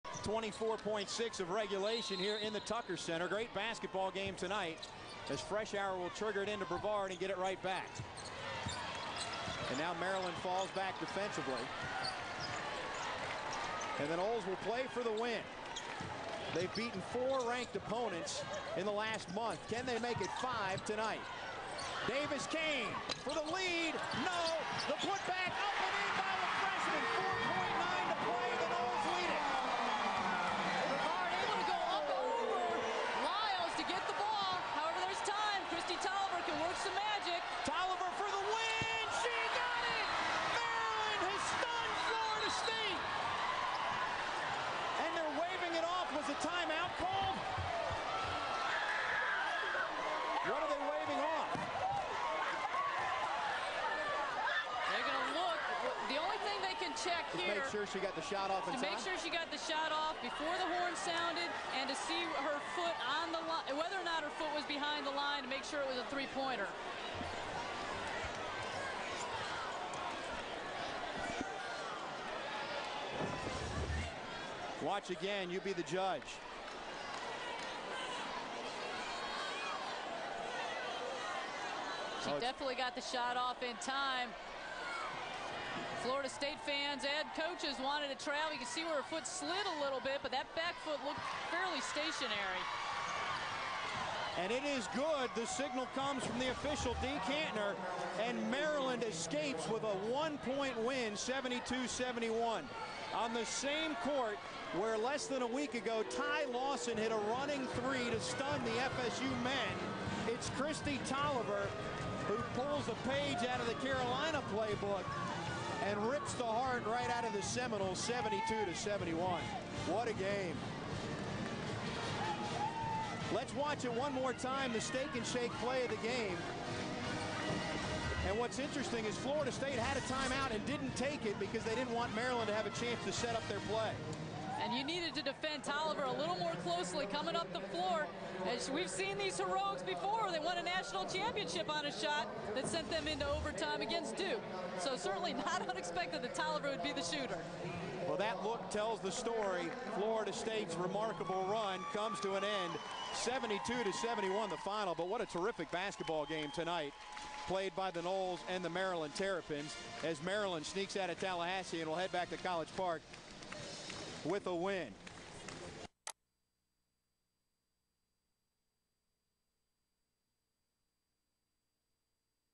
Kristi Toliver does it again! Maryland 72 at Florida State 71
On Feb. 2 at Florida State, the Terps' point guard took an inbounds pass with Maryland down two points and only 4.9 seconds on the clock, and dribbled to the 3-point line where she buried a trey at the buzzer to give the Terps a thrilling 72-71 win at previously ACC unbeaten Florida State.